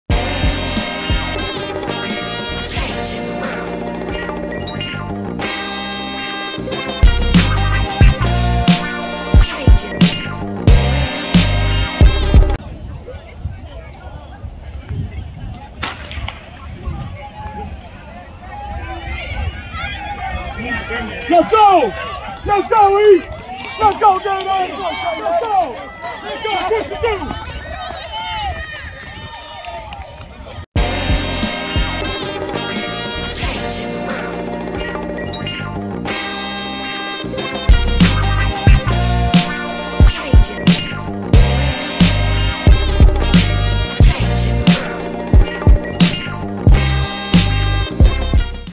High School 100 Meters Boys Heat 2
100 Meter dash between Buchtel vs East in A.P.S. 1st meet of the season that ended with a East victory in the rain.